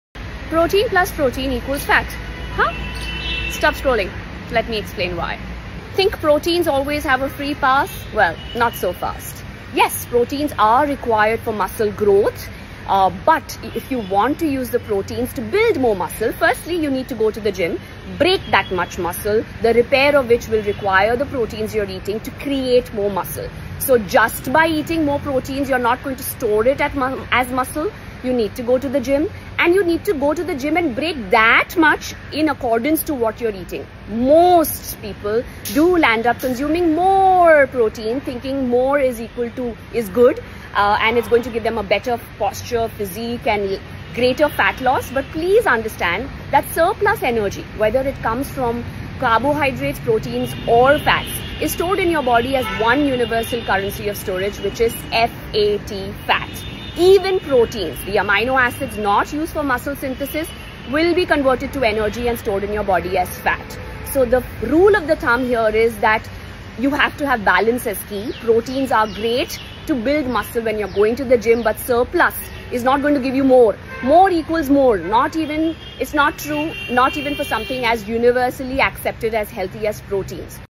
Nutrition Hacks Protein + Protein = Fat
Surplus protein can do more damage than good

Excess protein doesn’t just build muscle—it can turn into fat and put extra strain on your body.

A study by Antonio et al. (2014) that explored the effects of high protein intake on body composition, emphasized that when protein intake exceeds what the body needs, particularly without increased physical activity, the excess can be converted into fat or stress kidney function in the long term.

Another relevant study is from Paddon-Jones et al. (2009), which suggests that consuming protein beyond the body’s immediate needs doesn’t provide additional benefits and can contribute to fat storage if it results in an excess of overall caloric intake.